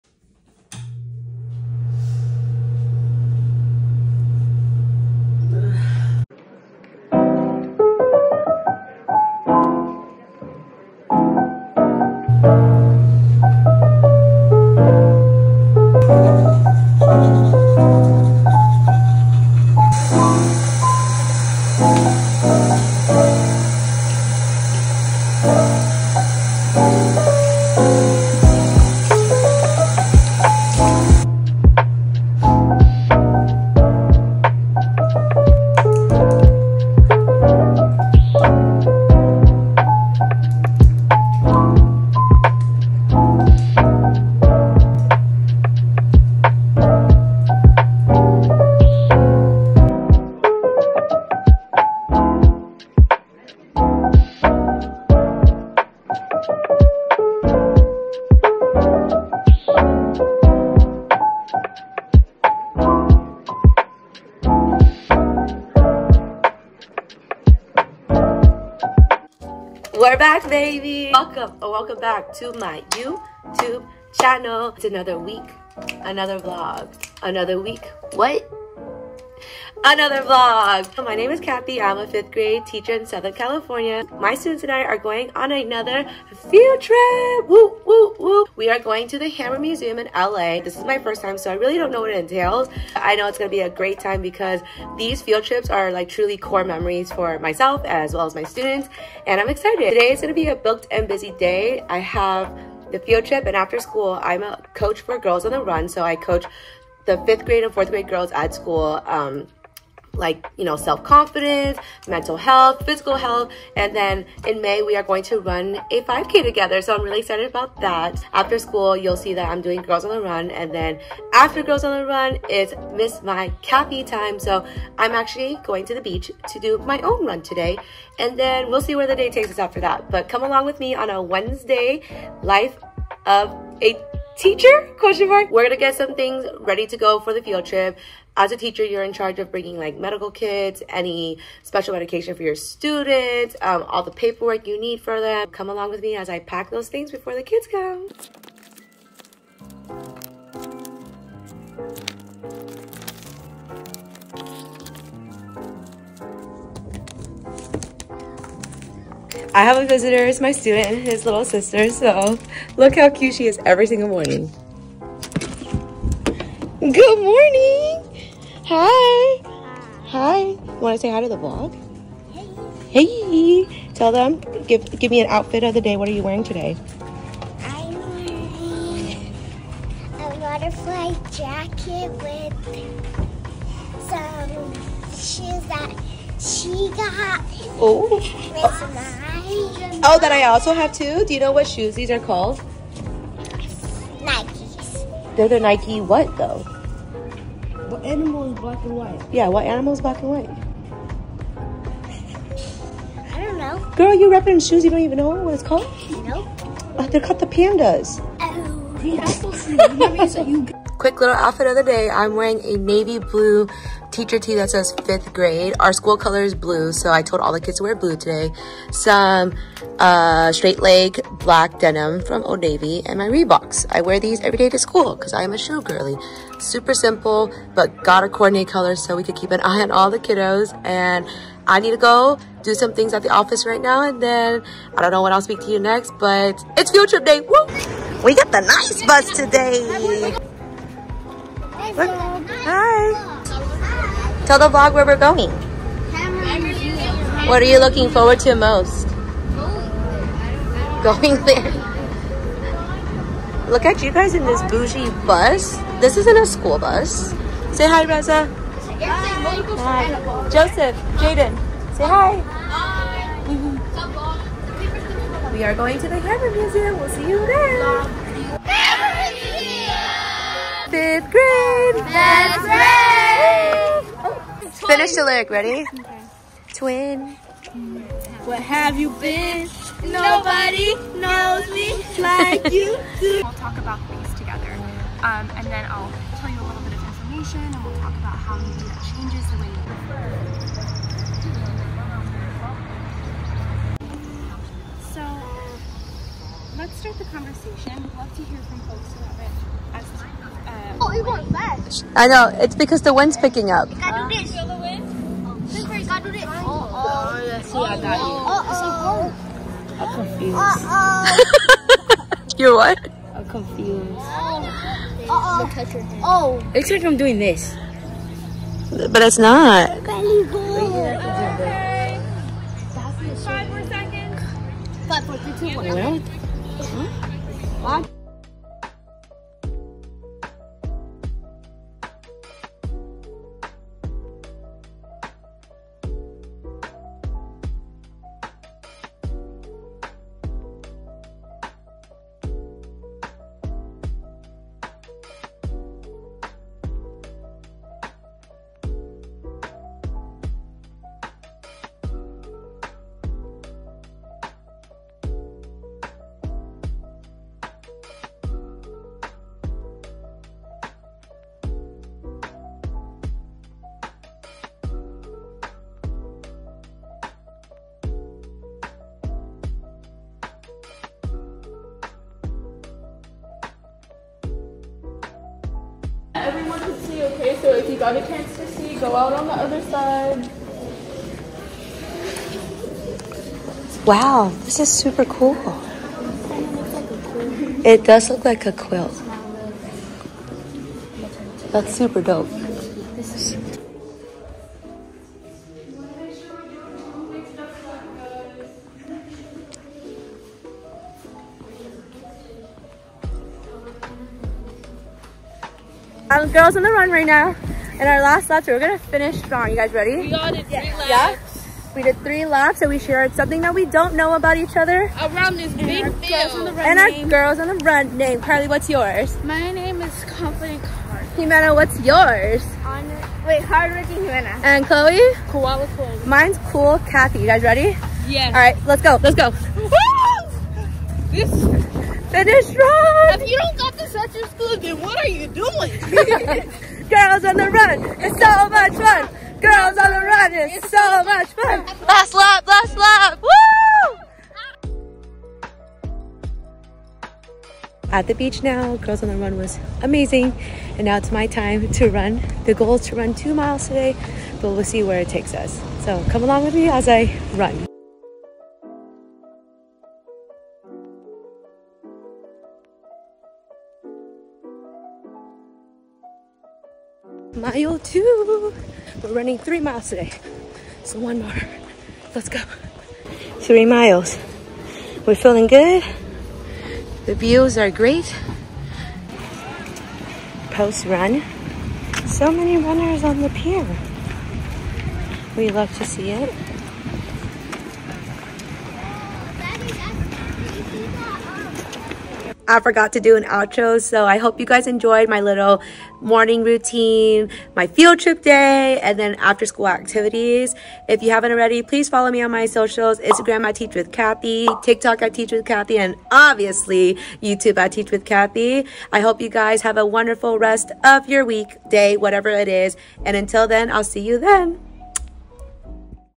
VLOG | morning routine, field trip, beach run!
My name is Cathy/Ms. Mai and I'm just a teacher living her best life ◡̈ ISSA ANOTHA WEEK, ANOTHA VLOGGG

Let's collab +connect!!